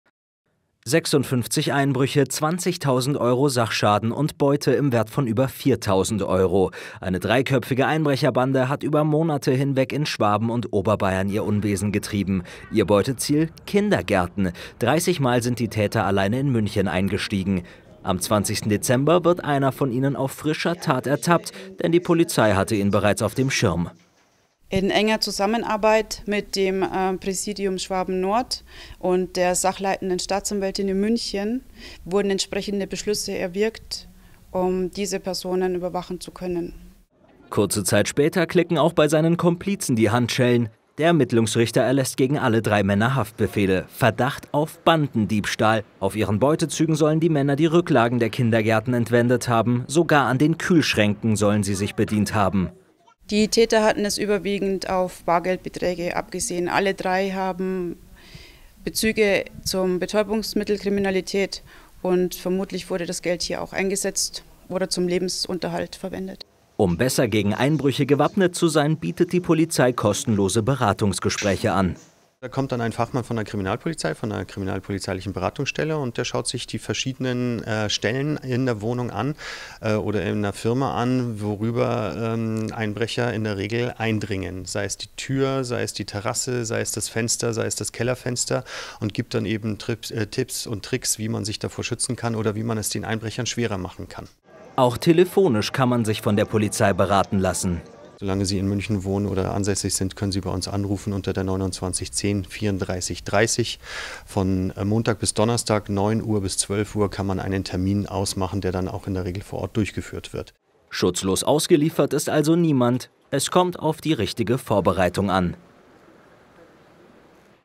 Kindergarten-Einbrecher - Polizei München fasst Verbrecherbande
Ein Kindergarten ist ein Ort zum Wohlfühlen.
Doch im vergangenen Jahr haben es besonders Diebe auf die sozialen Einrichtungen abgesehen.
Insgesamt 30 Kaffeekassen haben sie in Kindergärten in und um München gestohlen, die Kühlschränke ausgeleert und Scherben hinterlassen. Bei den Tätern handelte es sich um eine dreiköpfige Bande, die die Polizei nun fassen konnte. Lenny Peteanu berichtet.